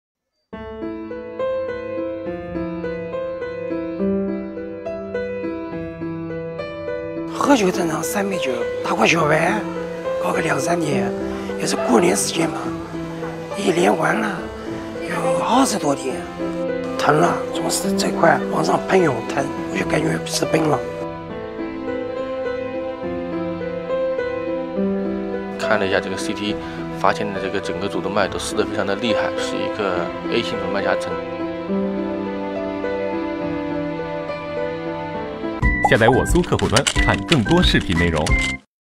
【29岁中国小伙连续熬夜20多天心血管破裂#】
【29岁小伙连续熬夜20多天心血管破裂#】江苏29岁的李先生患有高血压，最近连续熬夜20多天后，突然胸口剧烈疼痛，被紧急送医。经诊断，李先生患上A型主动脉夹层，是一种凶险的心血管疾病。病情危急，医院当即进行手术，李先生转危为安。提醒：心血管疾病有年轻化趋势，保持健康生活方式，注意预防。